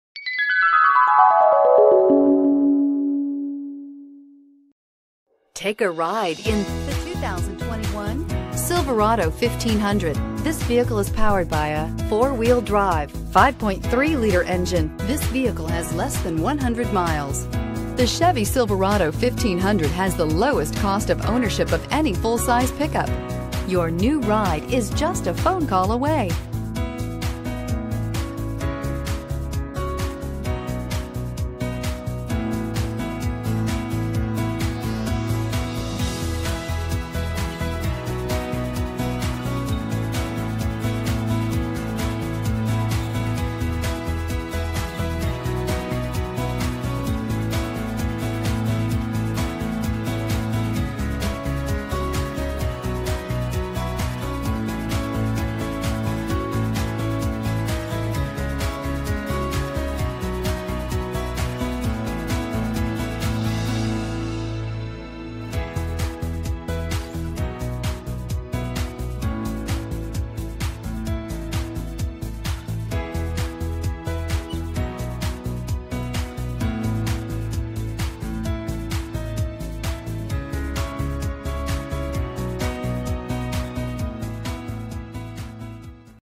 2021 Chevrolet Silverado 1T259266
2021 Chevrolet Silverado LT Trail Boss

Bill Gatton Chevrolet
1000 West State St

Silver Ice Metallic 2021 Chevrolet Silverado 1500 LT Trail Boss 4WD 10-Speed Automatic EcoTec3 5.3L V8 10-Speed Automatic, 4WD, Black Leather.Recent Arrival! Plus tax, tags and fees.  Price includes:$4250 - Chevrolet Consumer Cash Program. Exp. 03/31/2021